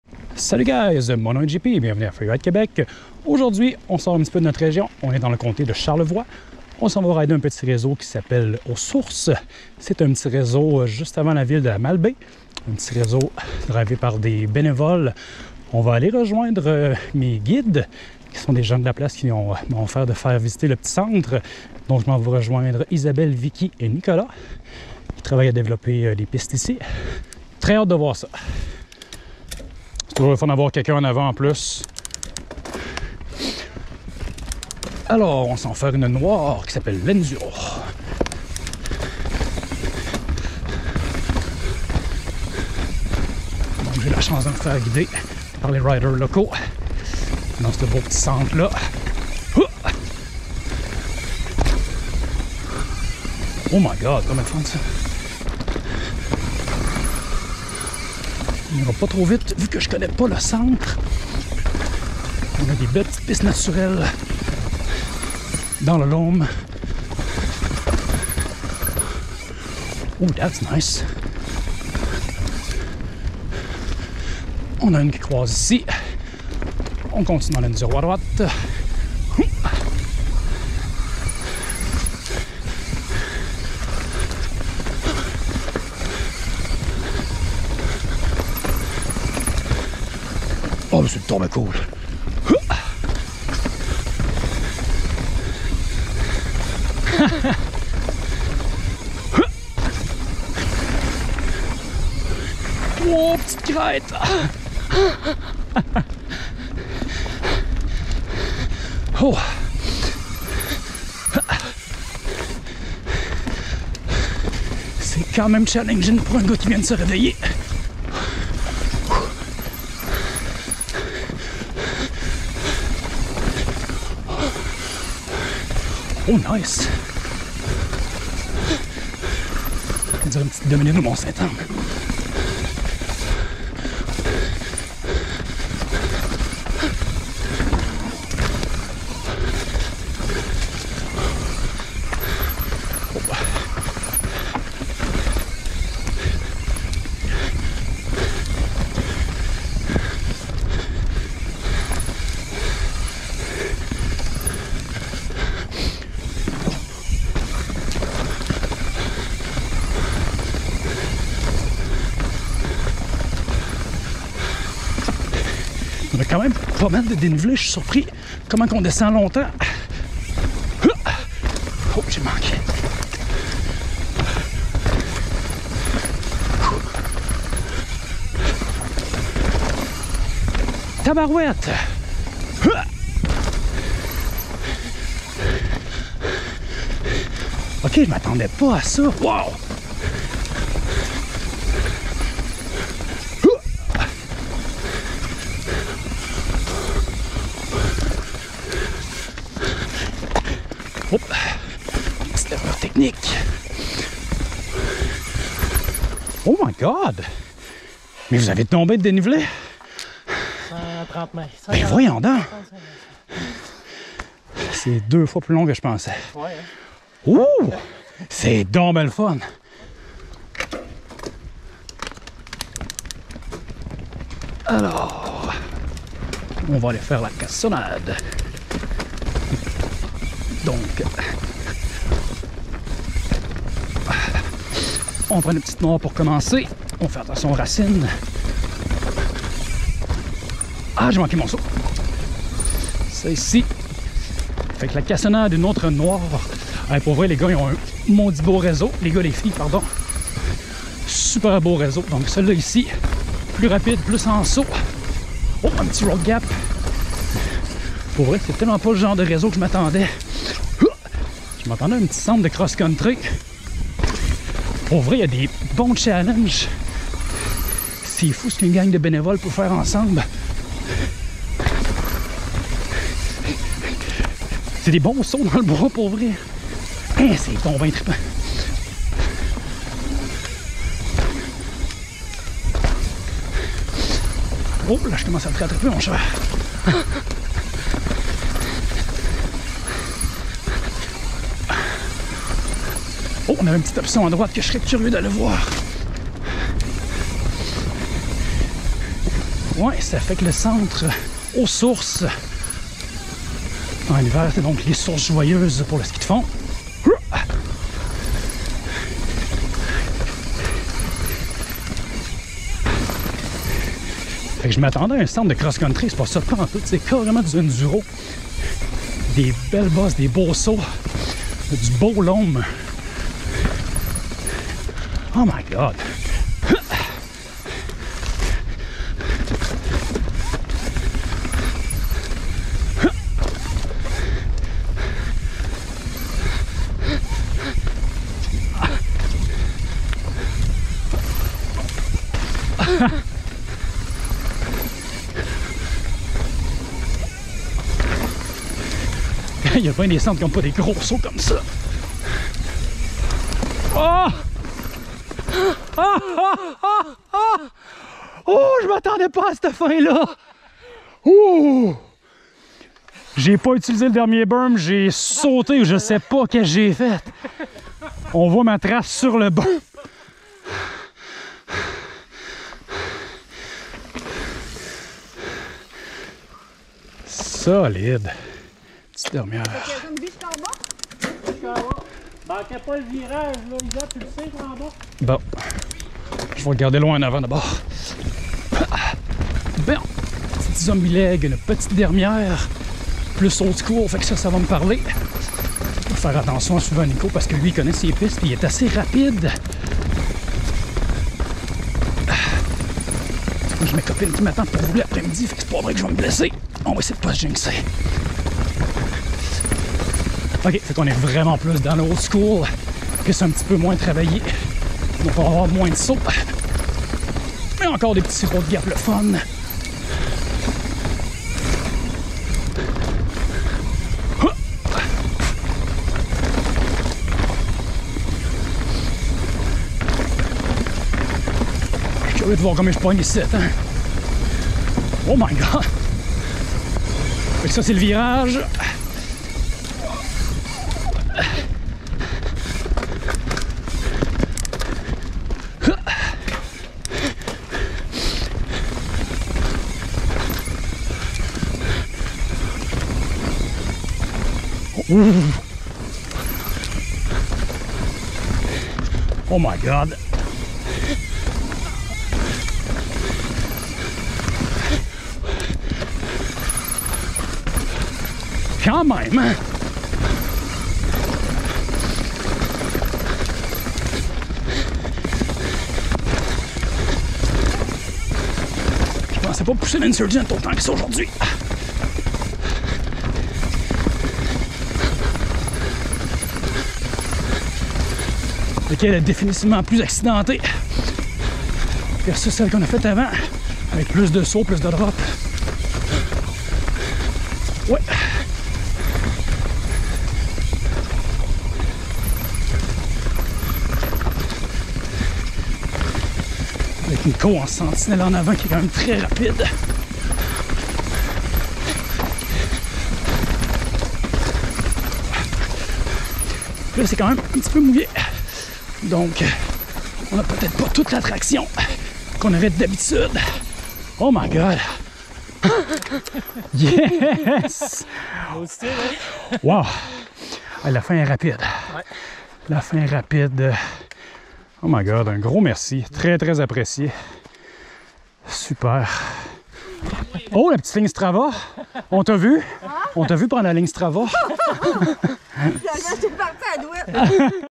Un petit GEM de réseau à découvrir dans Charlevoix!
*Visionnez en 1440p ou 4K pour avoir la plus belle qualité possible*
Trouvez cette trail sur Trailforks / Find this trail on Trailforks:

My bike:
-Evil Insurgent MX 2021

0:00 Intro
0:30 L'Enduro
3:41 La Cassonade
6:25 La Zombie Leg